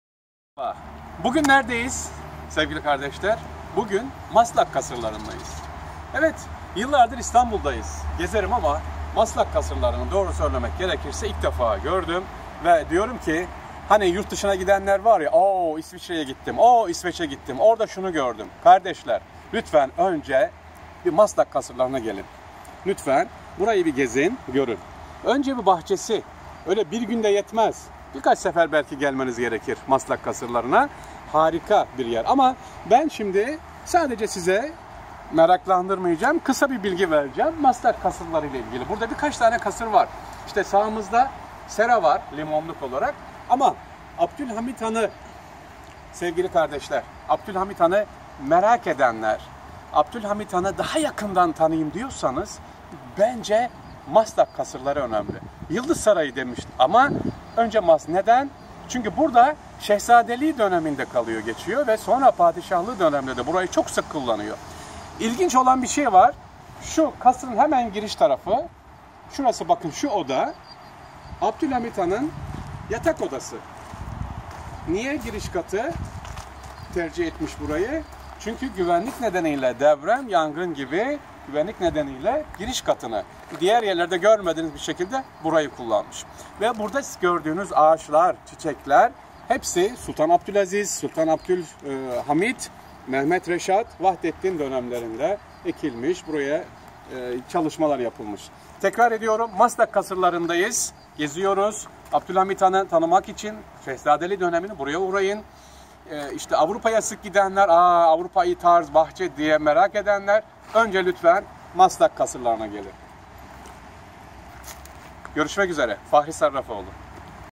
ŞEHZADELİKTEN SULTANLIĞA GEÇİŞİN MEKANI
(İstanbul'un Saklı Hikayeleri: 239 )
İstanbul’da görmemiz gereken o kadar güzel yerler var ki 30 yılı aşkındır İstanbul'u geziyorum ve keşfetmeye devam ediyoruz. İstanbul’un içerisinde 2.Abdülhamid Han'ın en çok sevdiği ve huzur bulduğu yerlerden bir tanesi de Maslak Kasırları.  Kimi kaynaklarda güvenlik nedeniyle Abdülhamit Han'ın giriş katında kaldığı söyleniyor, ama ben buna mütevazilik diyorum. Girişte hemen sağda Sultanın kaldığı oda var. Oldukça mütevazı ve bahçede bulunan yüzlerce çeşit ağaçlarla iç içe.  Fırsat bulduğunuzda mutlaka gidin derim.